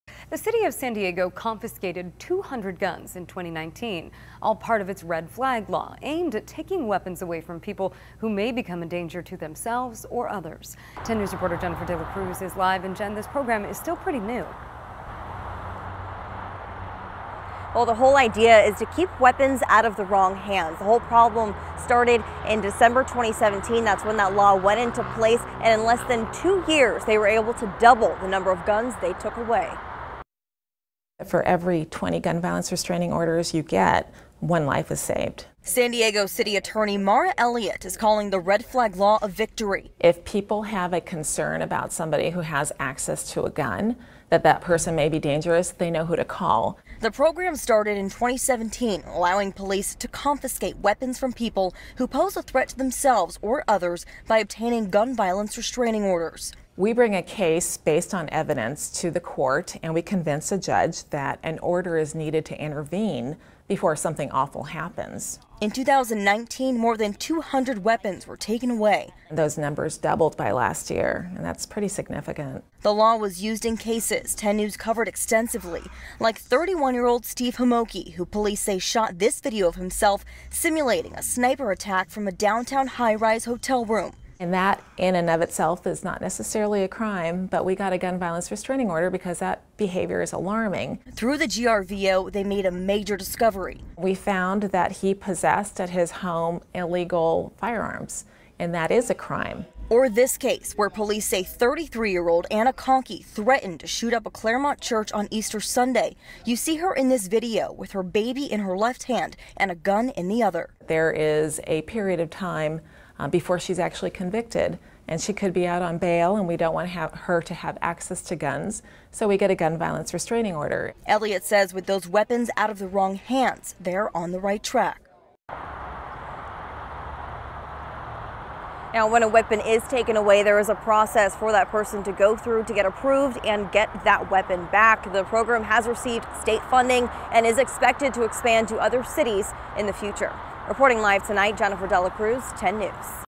Red flag law helps confiscate 200 weapons
The new Red Flag law has helped San Diego confiscate 200 weapons from those who pose a threat to themselves or others.